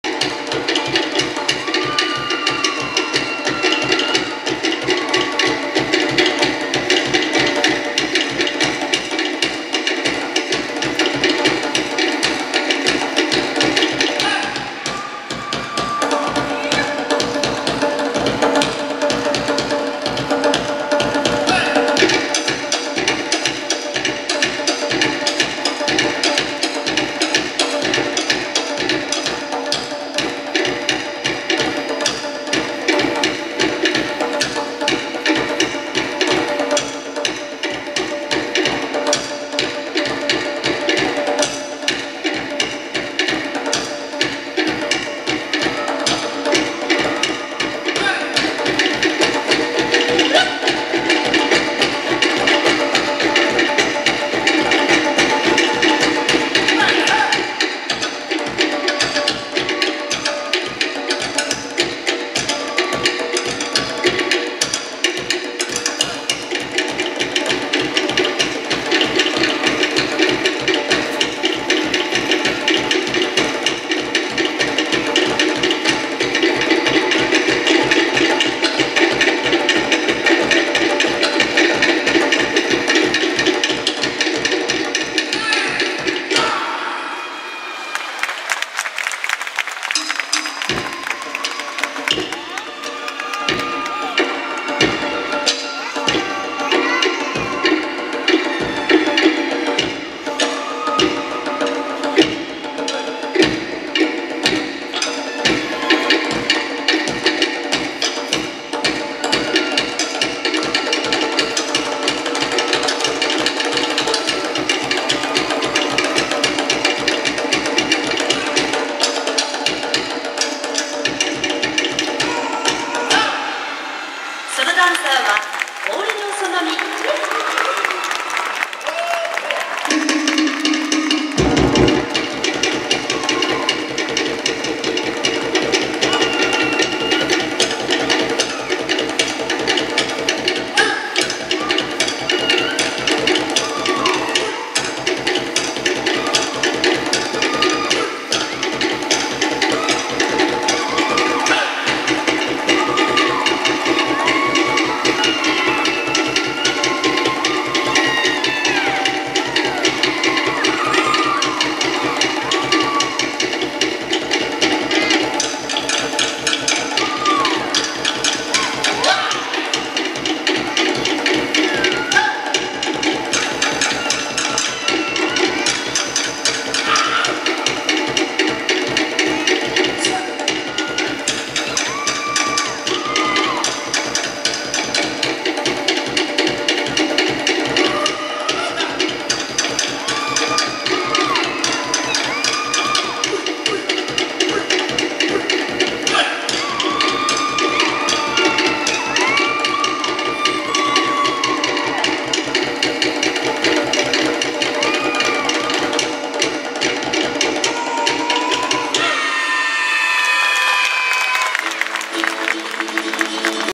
オテア スパリゾートハワイアンズ グランドポリネシアンショー オーリノ苑未
スパリゾートハワイアンズ　2014　グランドポリネシアンショー
オテア　otea　オーリノ苑未
Spa　Resort　Hawaiians